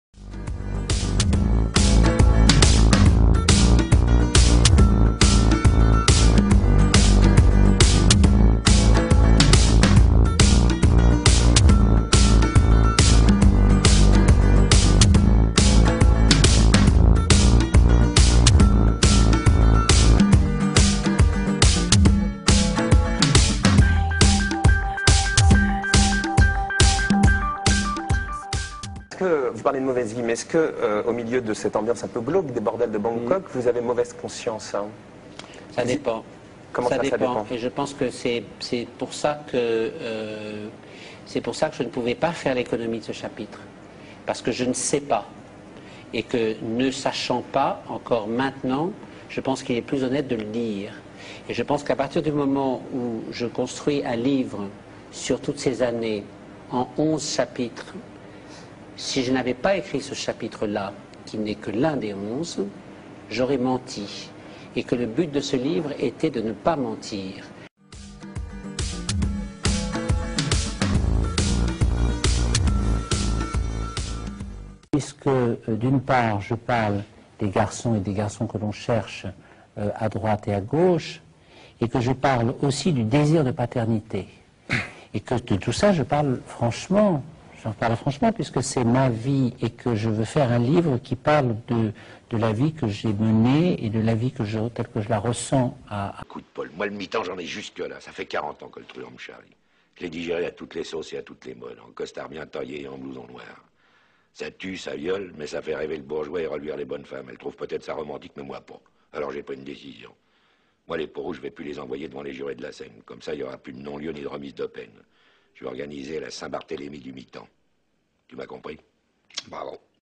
Frédéric Mitterrand nous parle de sa vie de pédophile
Il ne se cachent même plus.